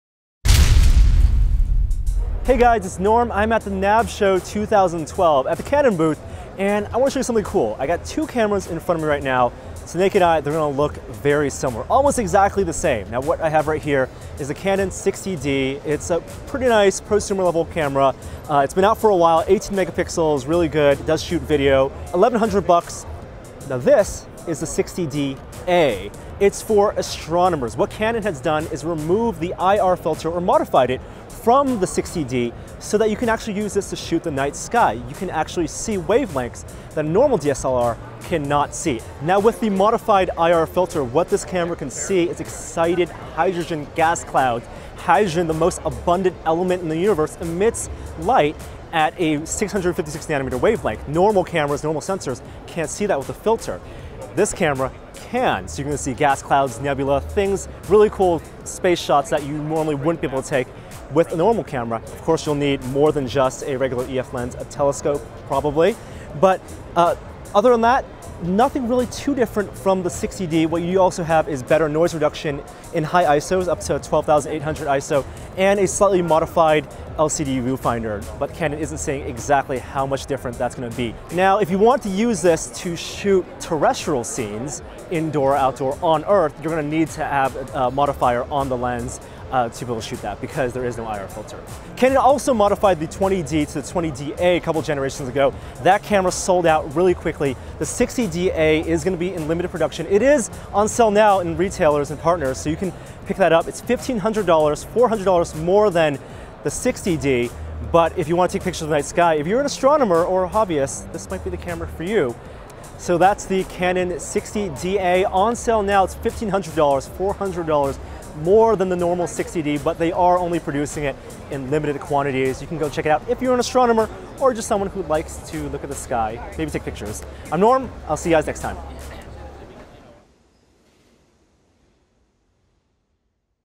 Canon EOS 60Da: A DSLR Made for Astrophotography
At NAB Show 2012, we inspect the differences between the Canon 60D DSLR and the limited edition variant that's built for astronomers.